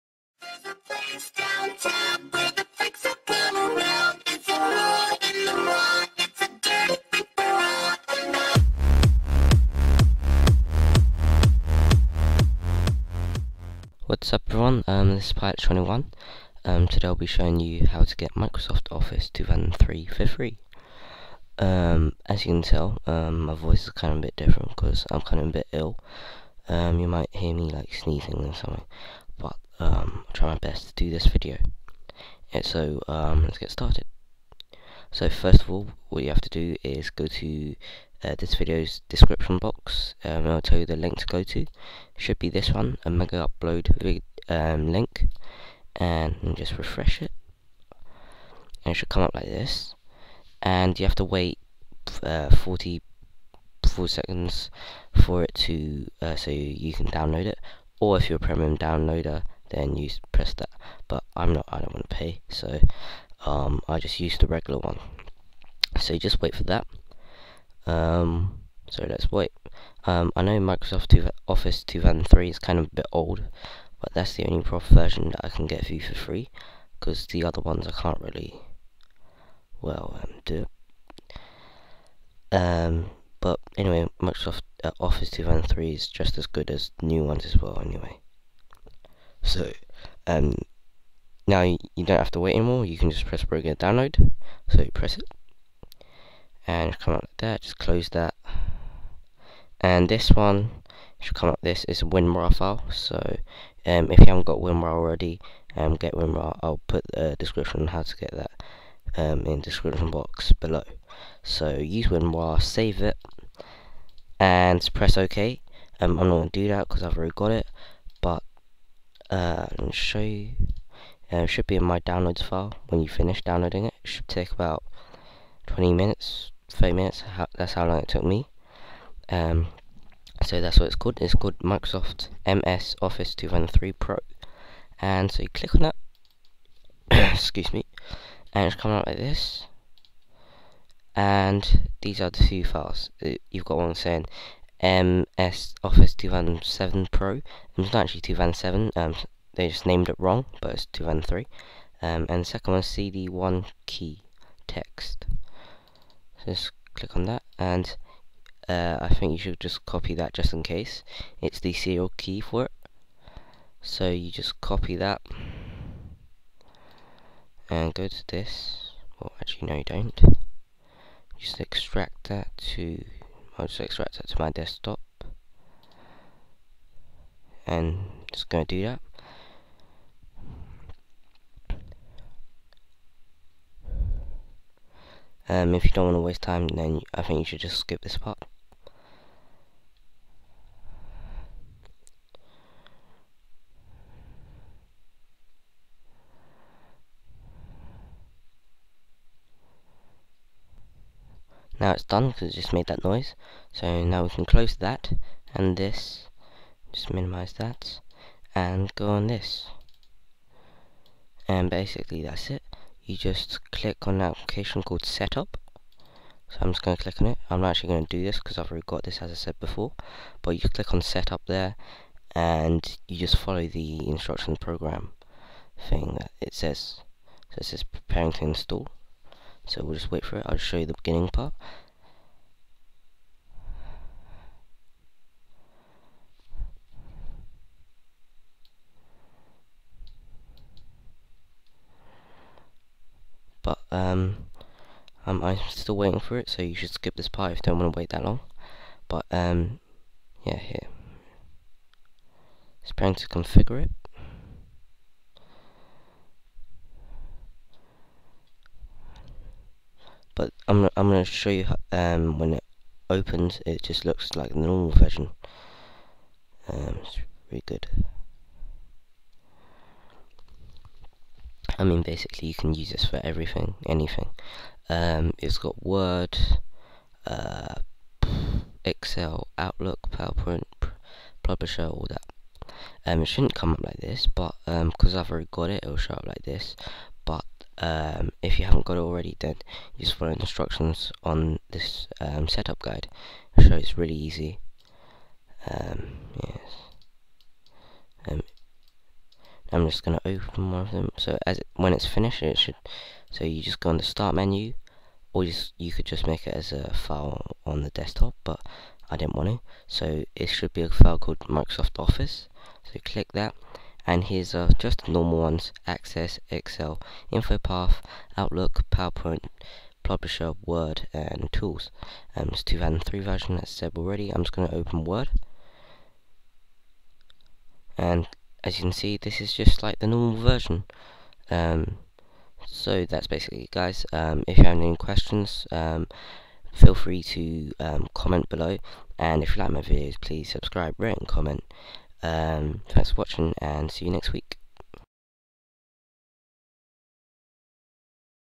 How To Get Microsoft Office 2003 For Free
Hello, This is Pillot21.

Today I will be showing you how to get Microsoft Office 2003 for free.

Pillot21 Productions Team